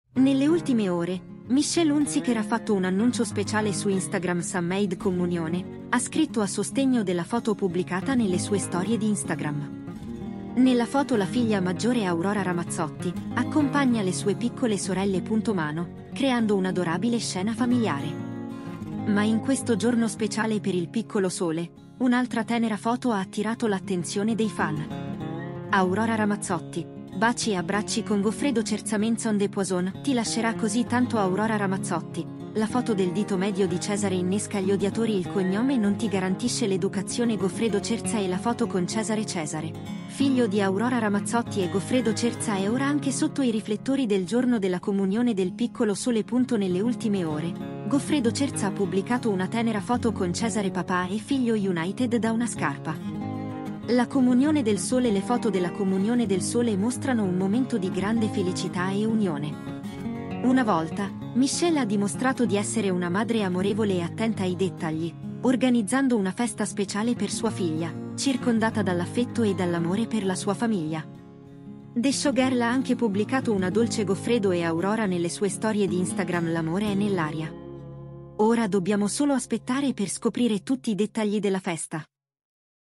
Aurora Ramazzotti, sua sorella Sole fa la comunione la Goffredo e Cesare sposta i fan
Nelle ultime ore, Michelle Hunziker ha fatto un annuncio speciale su Instagram "Sun Made Communione", ha scritto a sostegno della foto pubblicata nelle sue storie di Instagram. Nella foto la figlia maggiore Aurora Ramazzotti, accompagna le sue piccole sorelle.Mano, creando un'adorabile scena familiare. Ma in questo giorno speciale per il piccolo sole, un'altra tenera foto ha attirato l'attenzione dei fan. Aurora Ramazzotti, baci e abbracci con Goffredo Cerzaments on the Poison «Ti lascerà così tanto Aurora Ramazzotti, la foto del dito medio di Cesare innesca gli odiatori "Il cognome non ti garantisce l'educazione" Goffredo Cerza e la foto con Cesare Cesare, figlio di Aurora Ramazzotti e Goffredo Cerza è ora anche sotto i riflettori del giorno della comunione del piccolo sole.Nelle ultime ore, Goffredo Cerza ha pubblicato una tenera foto con Cesare papà e figlio United da una scarpa. La comunione del sole Le foto della comunione del sole mostrano un momento di grande felicità e unione. Una volta, Michelle ha dimostrato di essere una madre amorevole e attenta ai dettagli, organizzando una festa speciale per sua figlia, circondata dall'affetto e dall'amore per la sua famiglia. The Showgirl ha anche pubblicato una dolce Goffredo e Aurora nelle sue storie di Instagram "L'amore è nell'aria". . Ora dobbiamo solo aspettare per scoprire tutti i dettagli della festa.